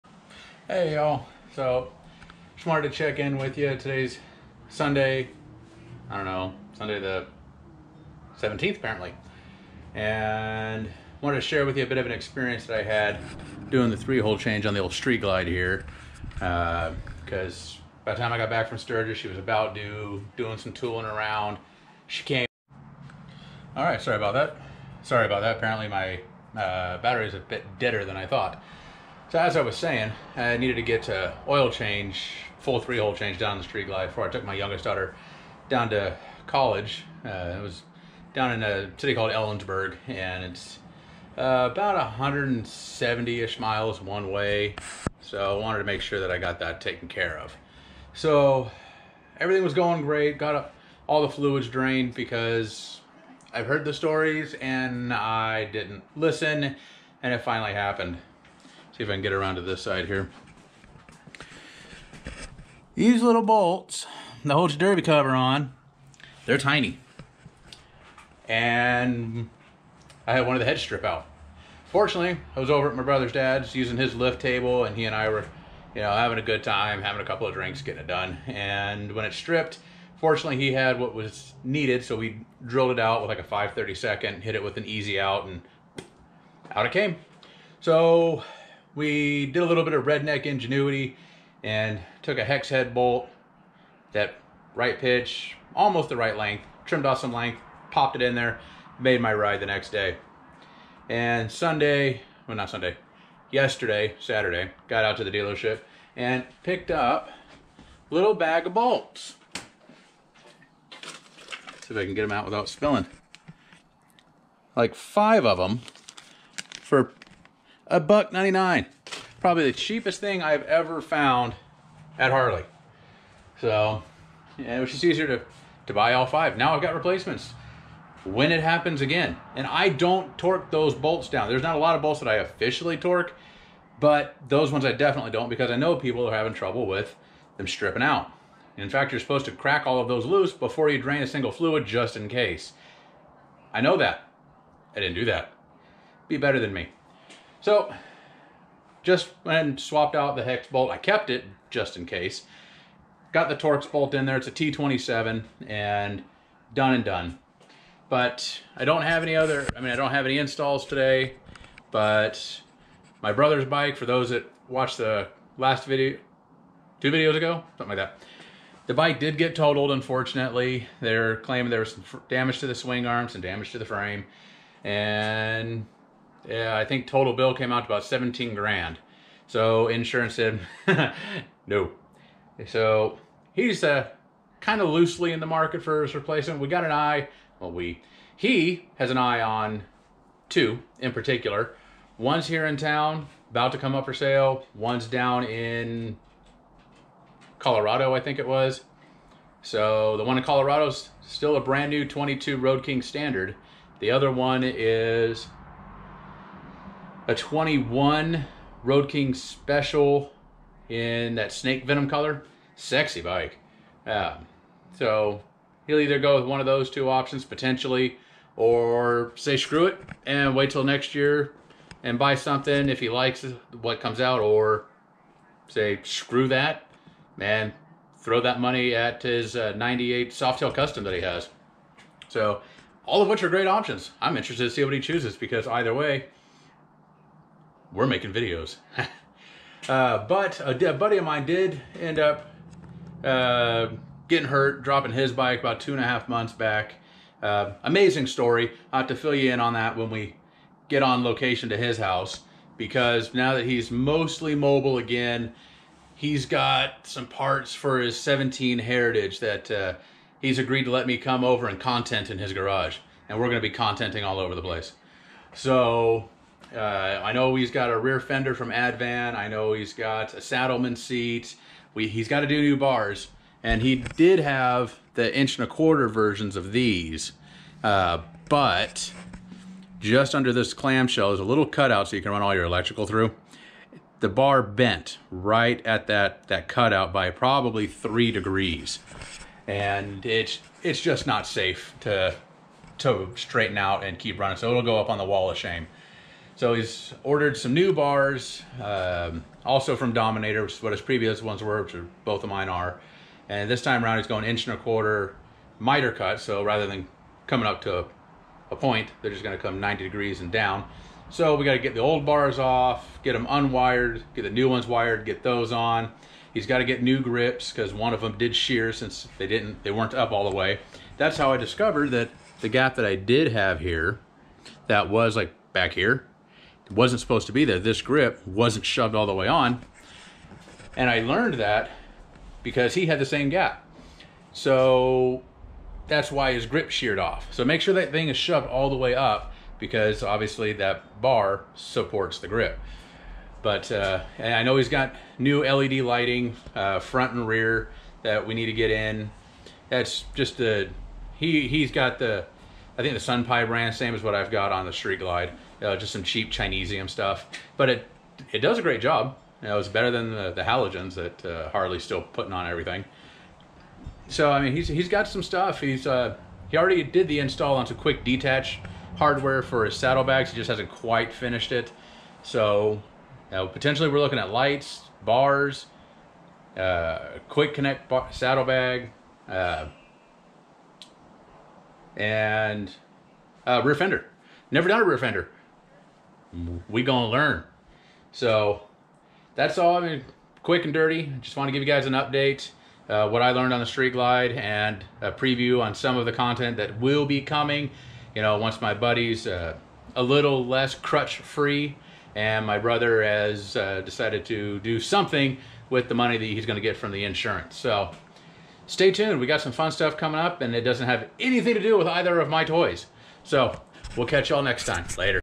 Street Glide frustration & new projects coming soon!
Derby bolts failing but new projects too!